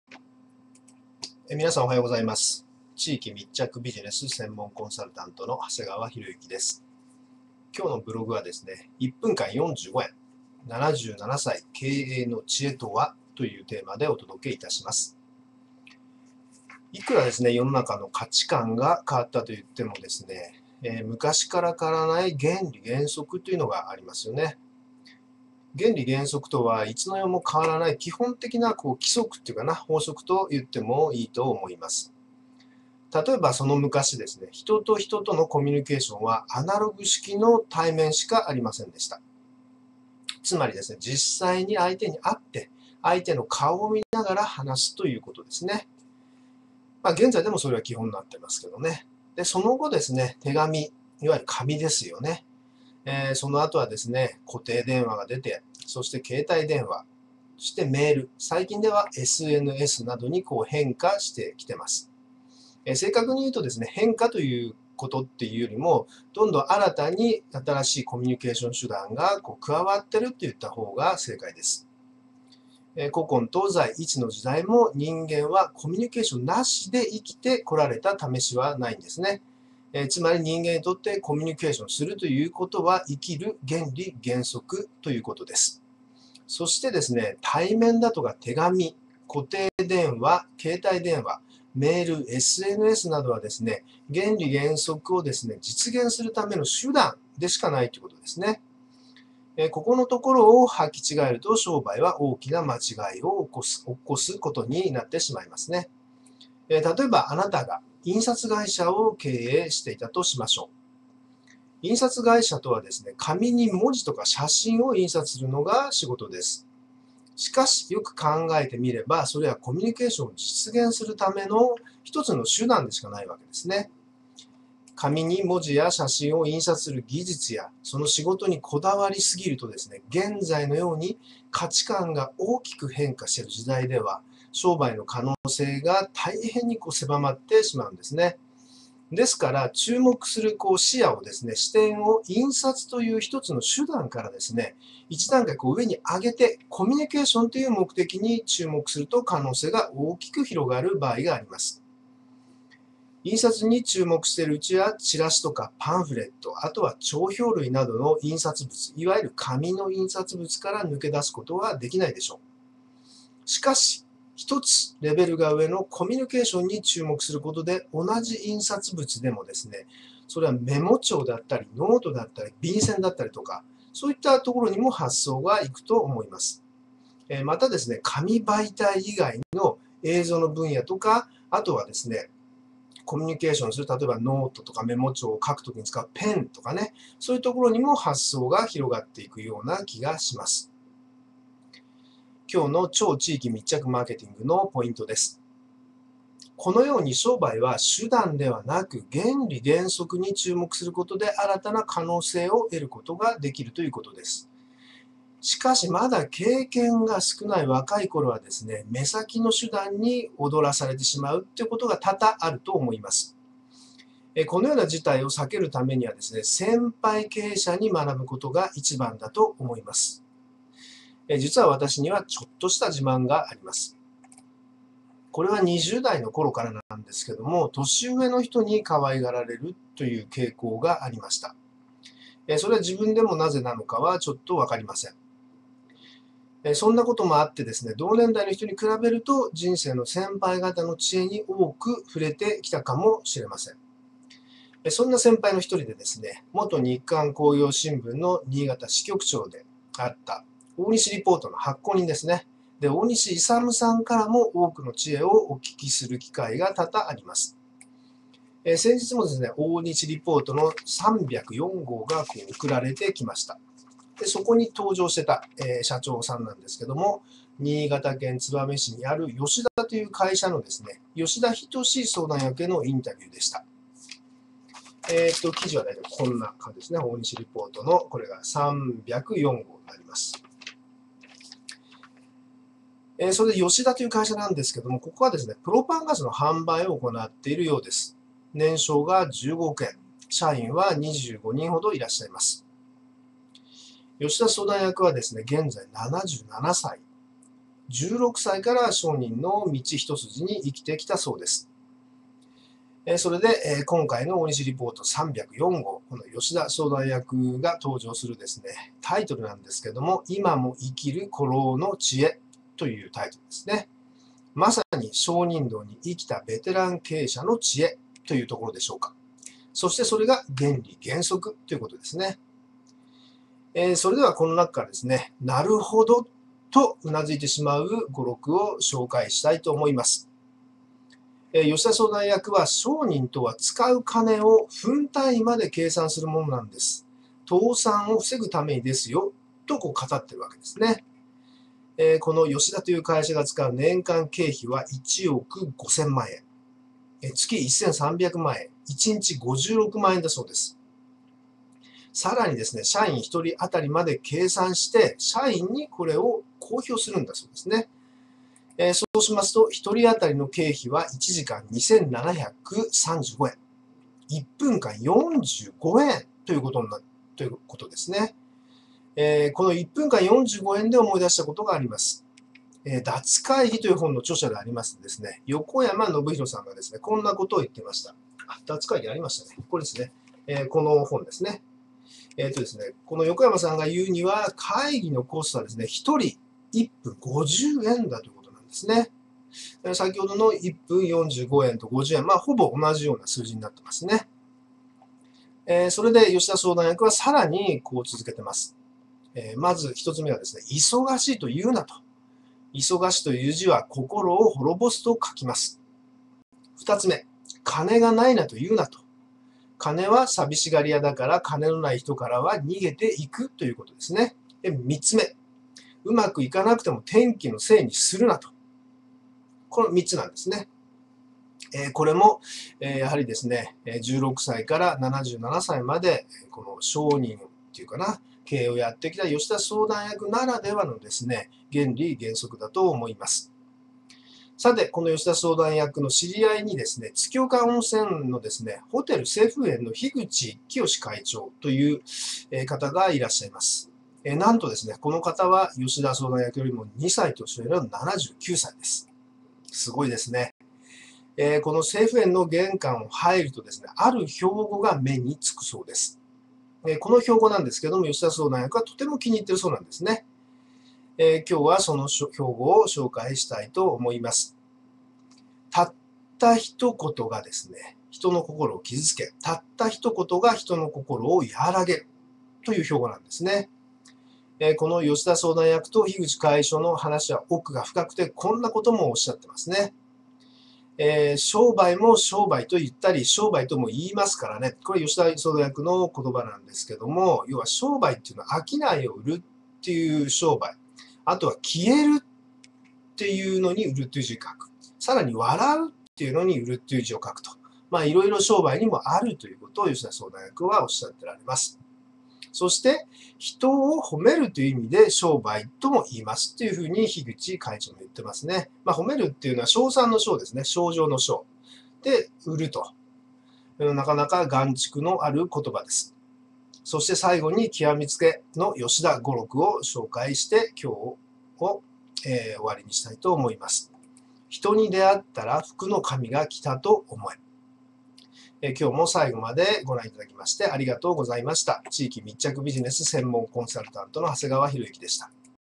■１分間４５円・・・７７歳、経営の知恵とは？（マーケティング）
「業績」を伸ばしながら、「人間性」も伸ばす秘訣は、いかにお客様に密着できるかがポイントです。超地域密着経営の仕組み創りと­­­­­­­­­­­­­­­­­­­­­­­­­­­­­­­­­­­­­­­­­­­­­­­­­­­­­­­­­­­­­­­­­­­­­­­­­­­­­­­­­­­­­­­­­­­­­­­­­­­­­­­­­­­­­­­­­­­­­­­­­­­­­­­­­­­­­­­­­­ヒ­ン­ト­を­お­届­け­し­ま­す­。
詳しくは、ブログ「感謝と喜びの超地域密着経営で、地域No.1を目指せ！」をご覧ください。